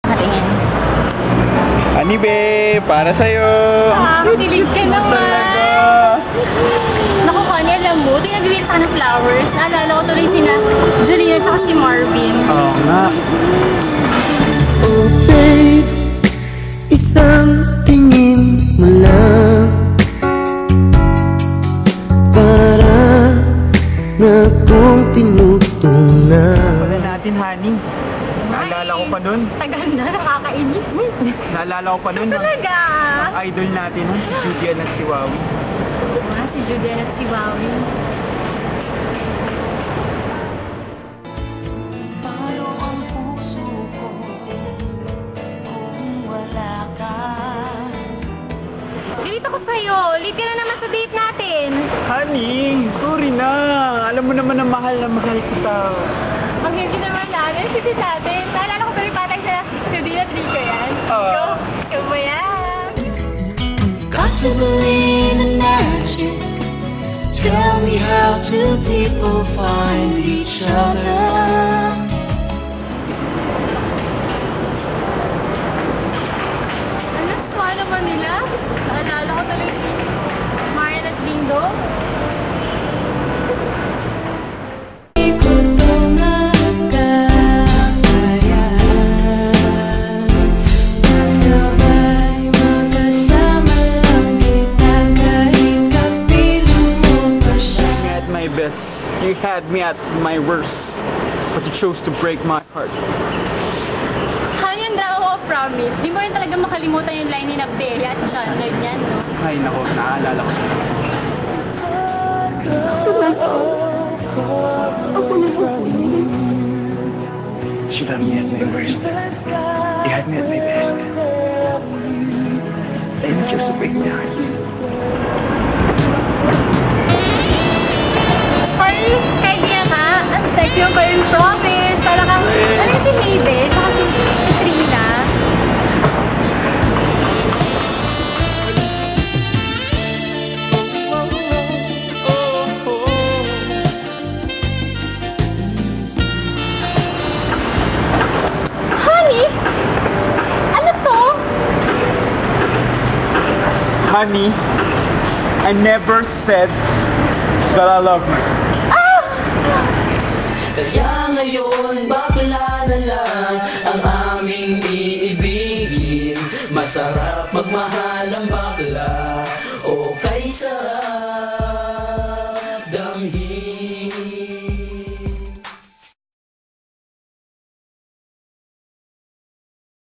The Top 7 Pinoy Love Teams!
Who are the top seven most famous pinoy love teams since the 90's? Check them out here! Kindly rate our video too! Thanks!